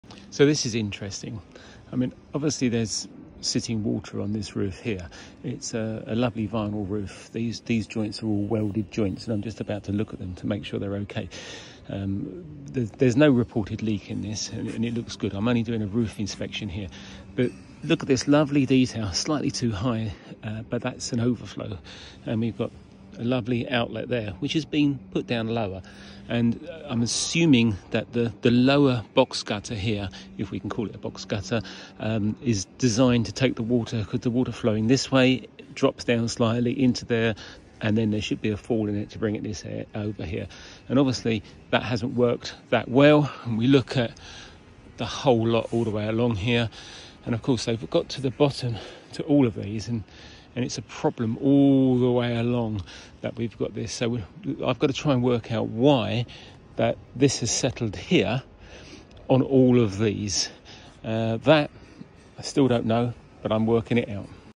New Flat Roofs: Why are they holding water?
Steven Dickinson
London Flat Roofing

If you look at these roofs on this block each one of them has got a problem where the falls are insufficient now when we look over the top part of the roof there is a good fault but as we get to the bottom it seems to disappear I'm trying to work out why this would be taking that as they are new builds and the falls of the roof would've been probably made using some sort of tapered insulation. Why does it all of a sudden flatten out when you get this last section? Well I was doing a roof survey on this particular roof and then I looked at the rest now I have to do an invasive inspection into this roof to see if I can find out why.